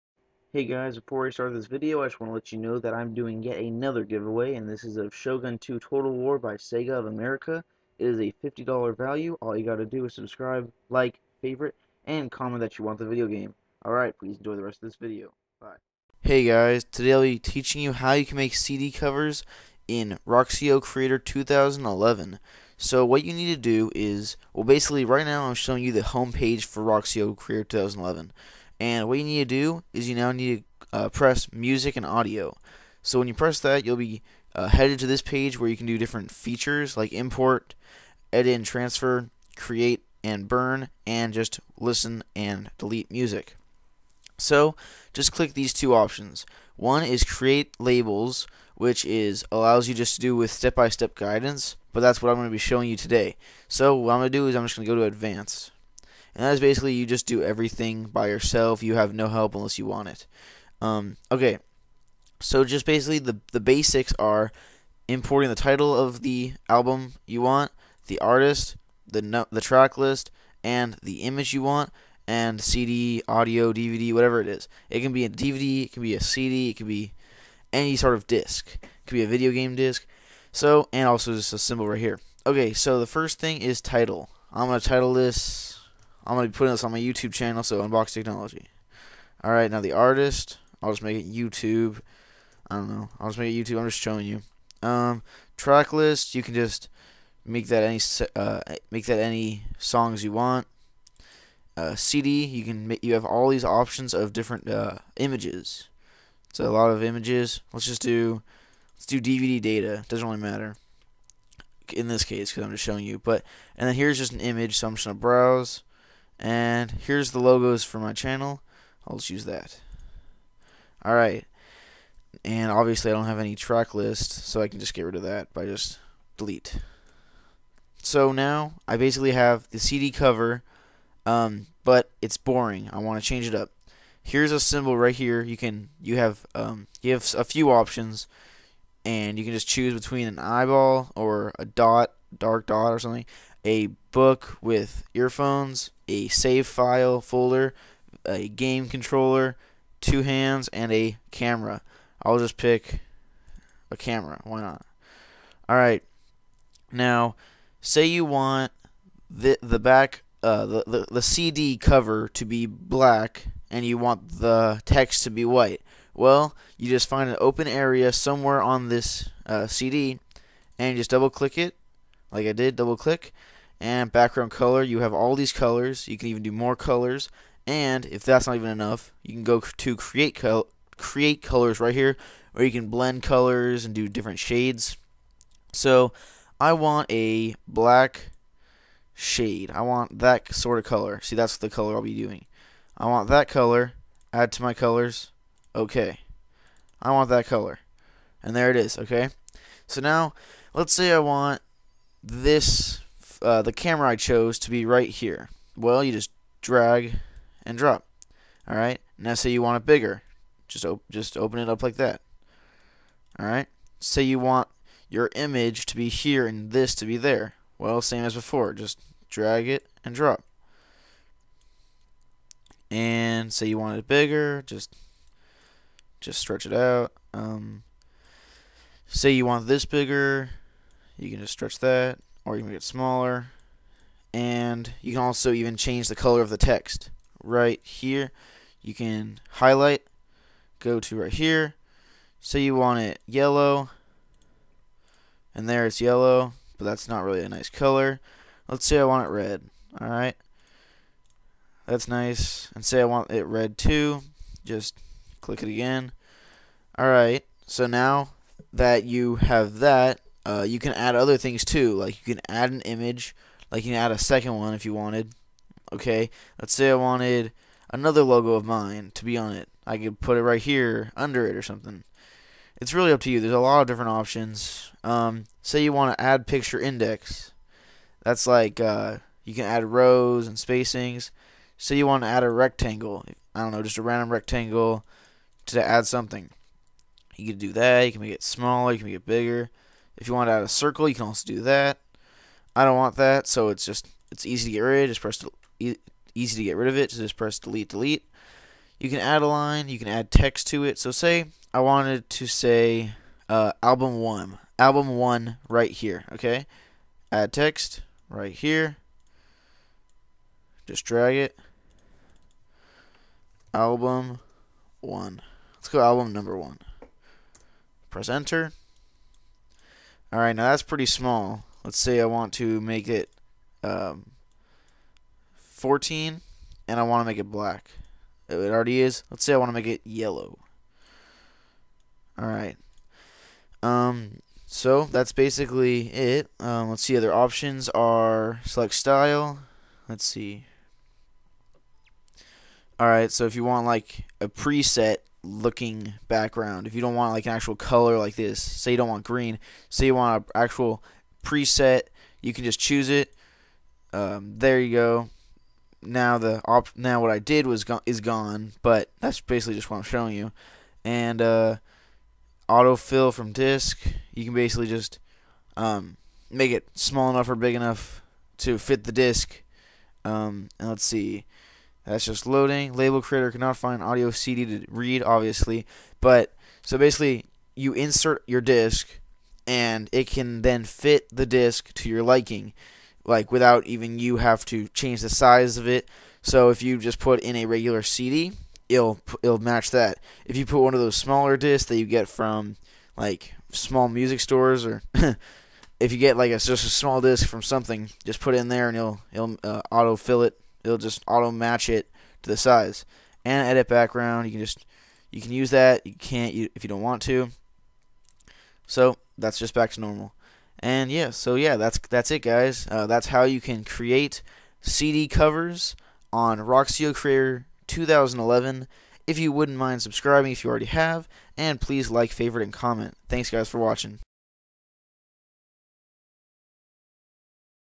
How To Make CD/DVD Covers On Roxio Creator 2011 + GIVEAWAY (OVER)
I show you exactly how to make customized disk covers on Roxio Creator 2011. I also am giving away Shogun 2: Total War by Sega of America. If you want more information about the game, check out